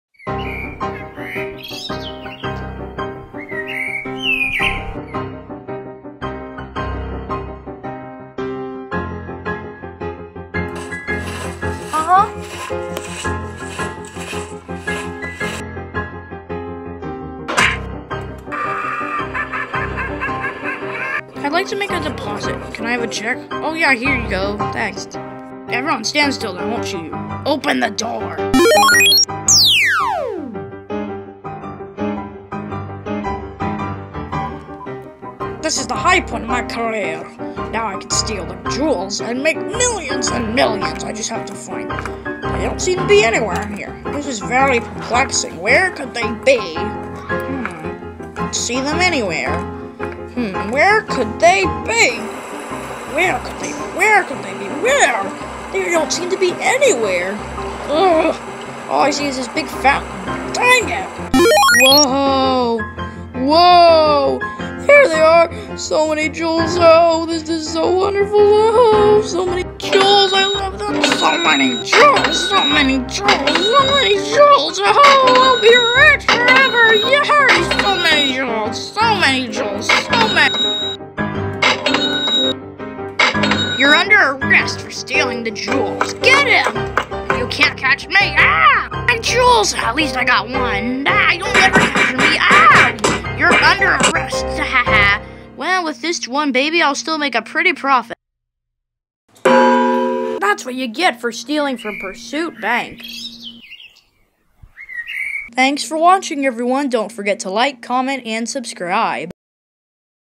Lego Bank Robbery
A treacherous villainous crook tries to steal the Jewels of The Rich